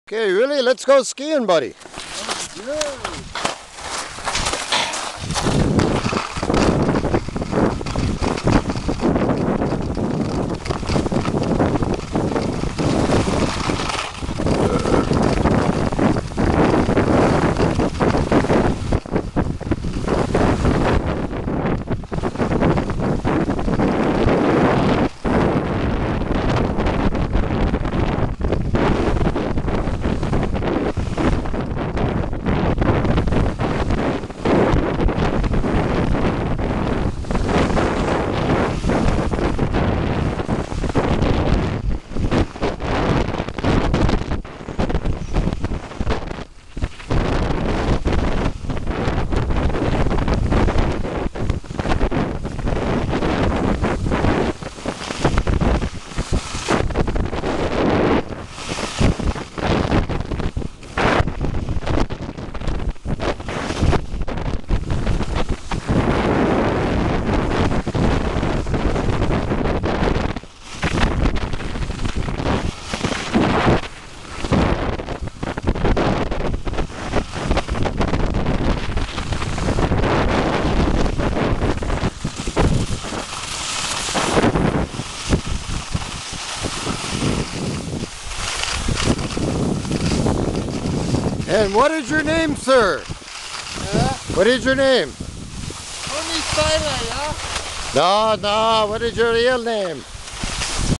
uli skiing 016.AVI at grouse mountain today
Uli Metz From Freiberg Barvaria ..  Skied to school every day as a child .. came here and skied for a few years then Life happened and he stopped .. This is this thrid time on skis after a 25 year absence ,.. He's still got it . Hup Hup Hop Hop Hop  Cow bell Cow bell ..    zoom zoom .